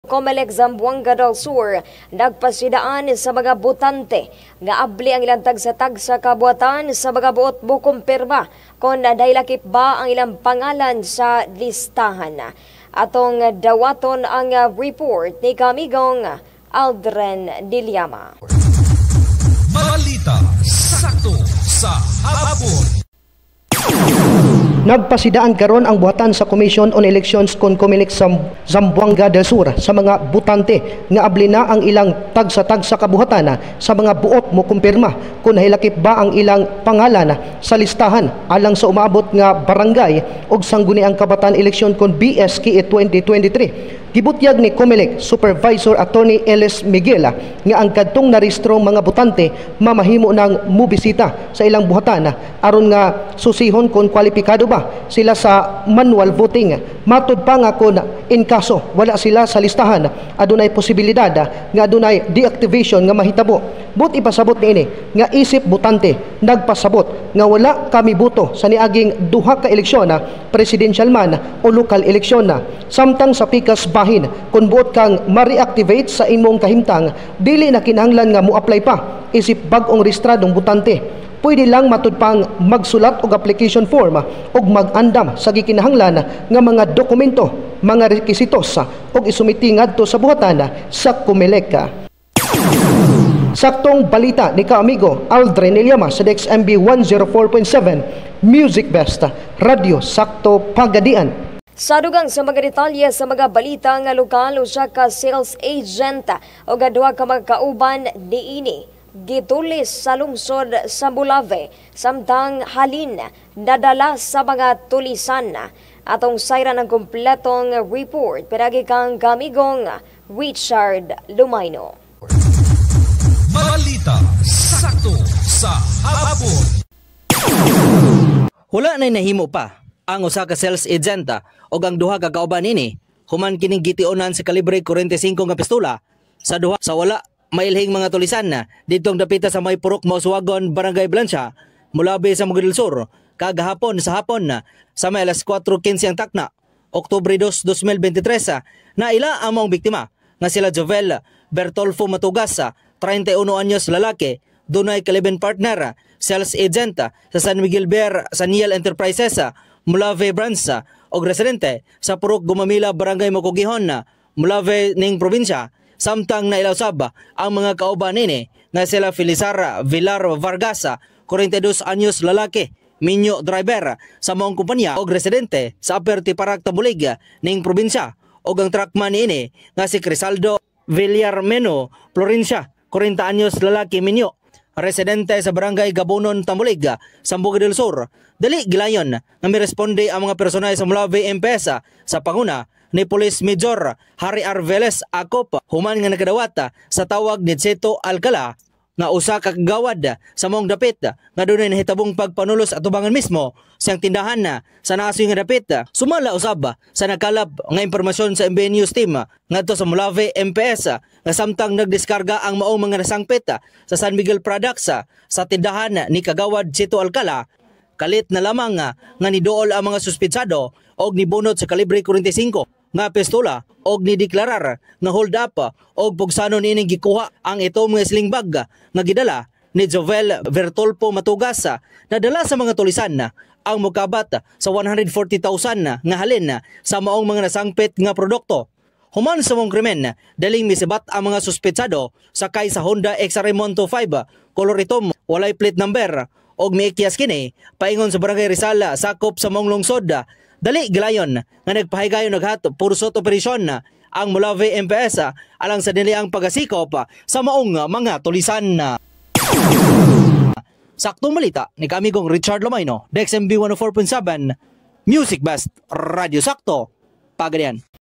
HIGHLIGHTS | LOKAL NEWS | OCTOBER 3, 2023
1. COMELEC Zamboanga del Sur nagpasidaan sa mga botante nga abli ang ilang tagsa-tagsa ka buhatan sa mga buot mokumpirma kon nahilakip ba ang ilang pangalan sa listahan.

2. Usa ka sales agent ug duha ka kauban niini gitulis sa lungsod sa Molave; halin nadala sa tulisan.